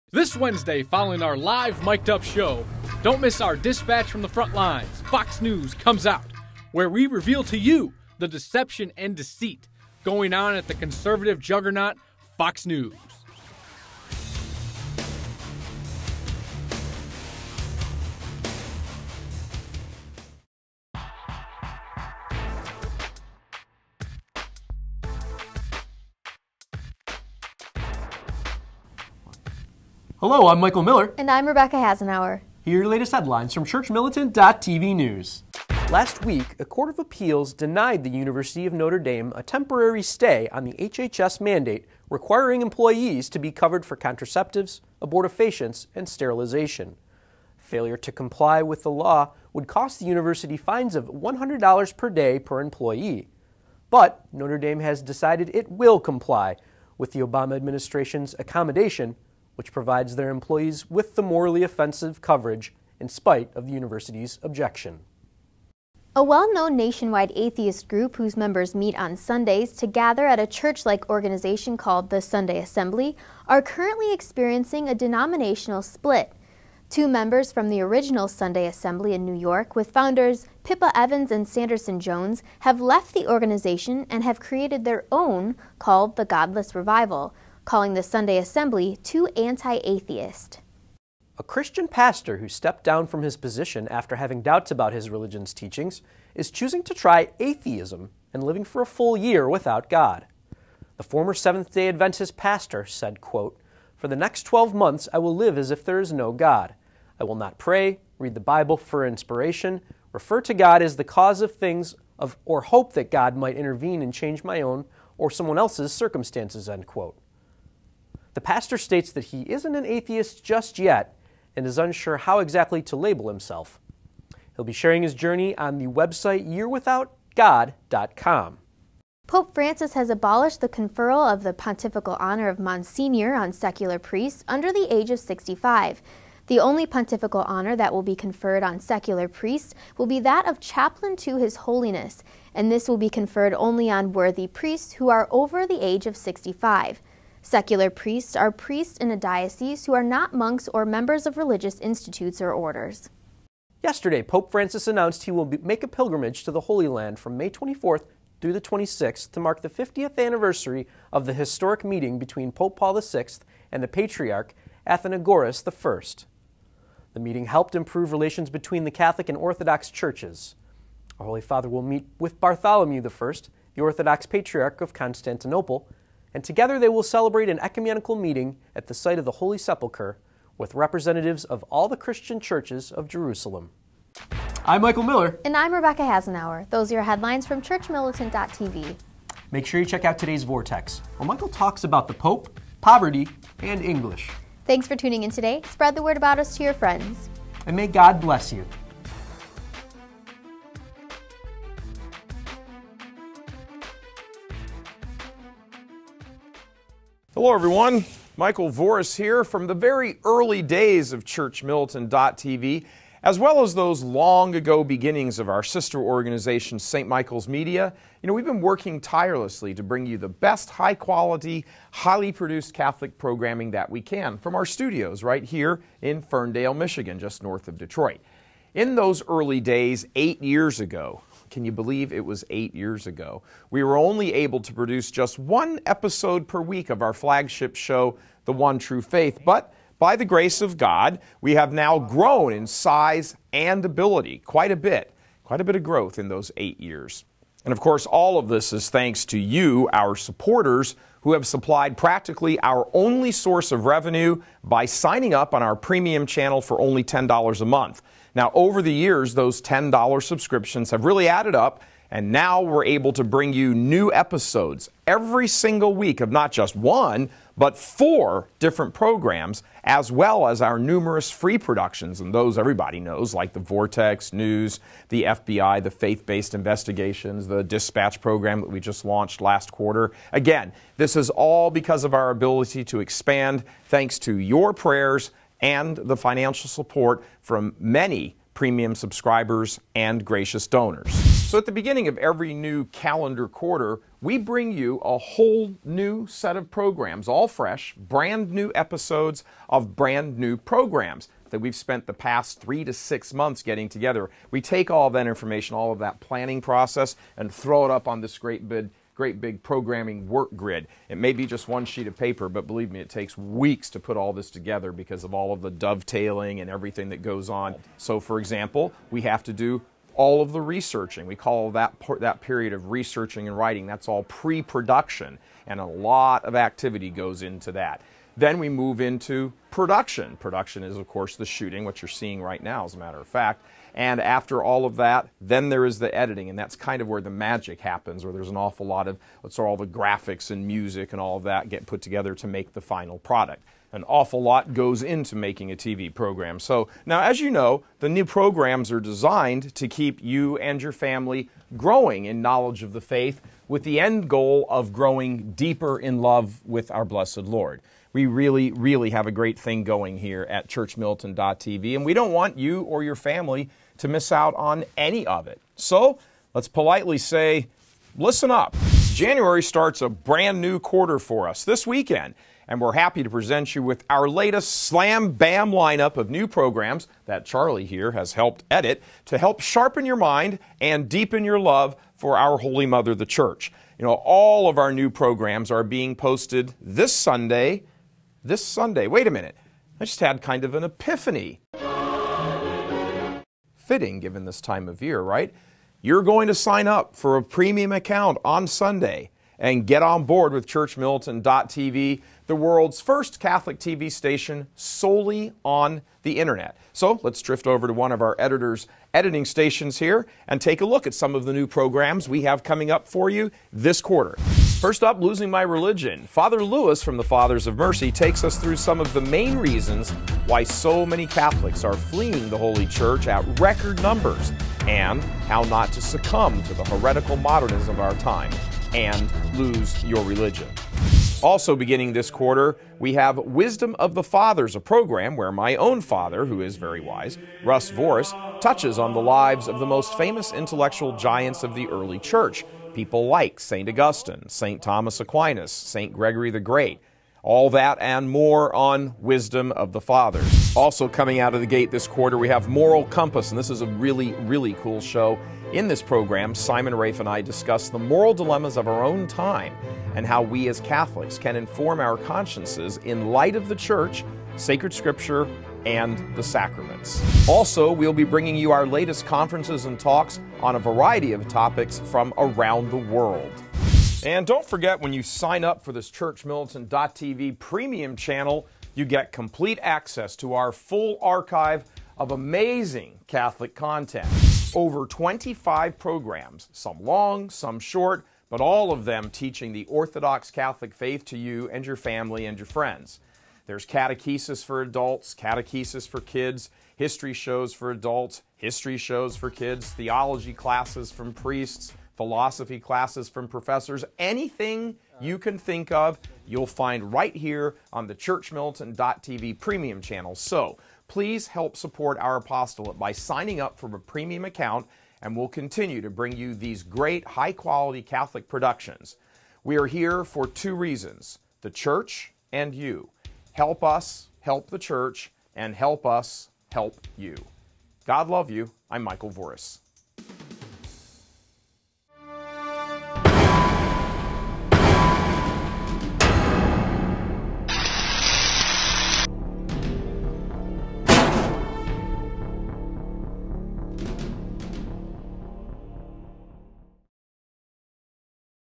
ChurchMilitant.TV News 01-06
Today's stories -
Notre Dame Denied HHS Extension
Atheist Denominational Split
Pastor Turns Atheist For One Year
Pope Abolishes Clerical Title
Holy Father To The Holy Land